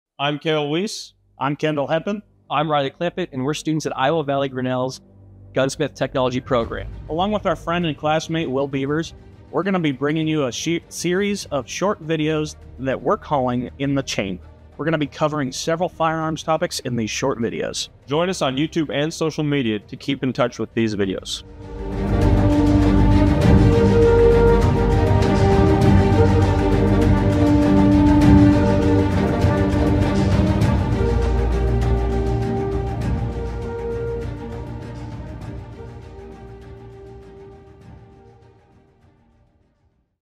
Introduction- "In the Chambers" //Iowa Valley Grinnell
Welcome to "In the Chambers" with Iowa Valley Grinnell. Join fellow students as they embark on a journey through Gunsmithing, tips and tricks, and more!